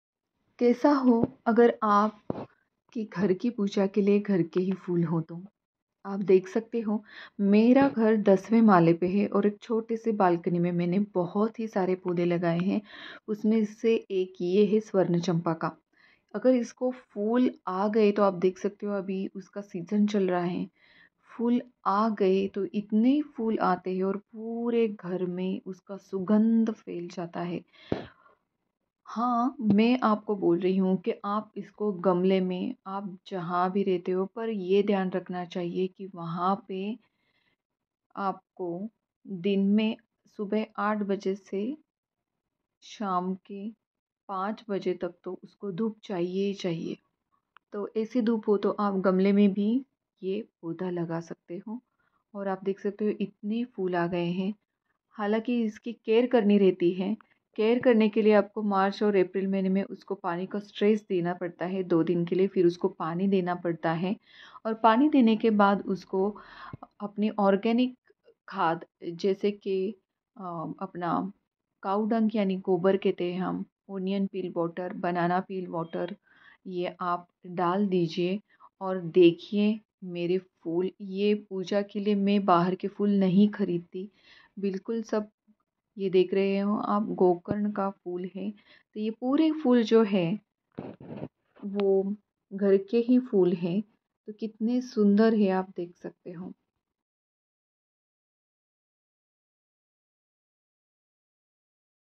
My fully bloomed swarn champa plant
How to make swarnchampa bloom?

fertilizing swarn champa plant is very very important.
Make sure to feed it properly as if it is in a pot.
and It should be under direct sun about 5-70hrs daily.

cow dung+onion + banana peel water is best fertilizer.